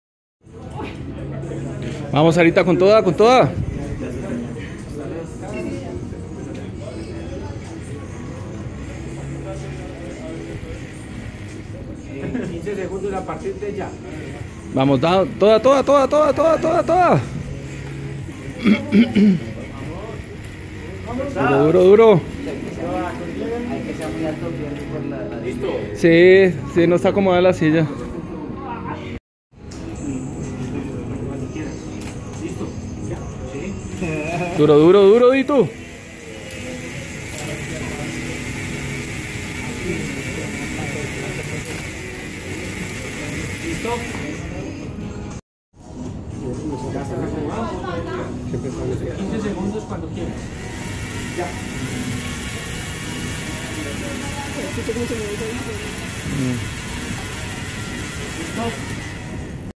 Conoce tu Potencial al Pedalear - Totto
Totto conoce tu Potencial al Pedalear, miércoles 9 de Agosto, Totto Flagship Store Zona T Bogota Calle 83 #12-49 Bogotá. Evento realizado el dia 9 de Agosto en la zona T Bogota, conociendo el potencial al pedalear solo midiendo la cadencia.